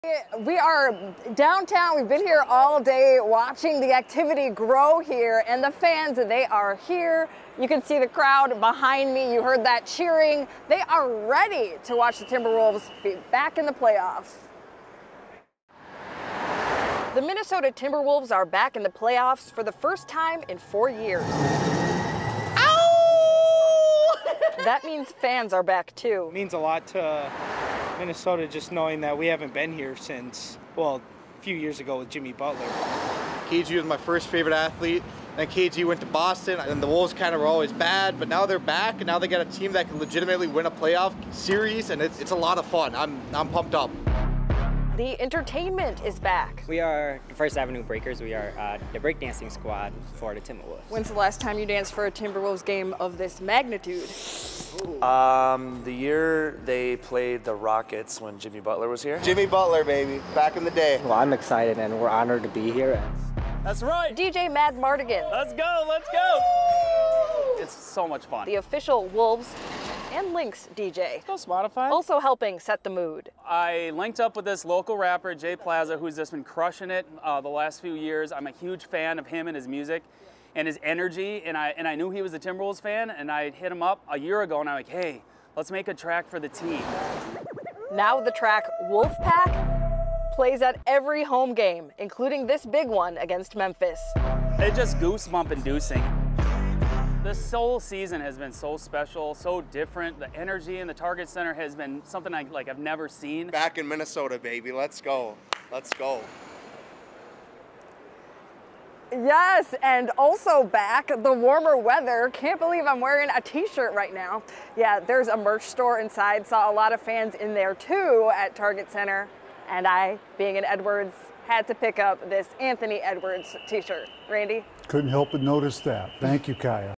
Wolves fans turn out for Game 3 against Memphis
The Minnesota Timberwolves are back in the playoffs for the first time in four years — which means fans are back, too.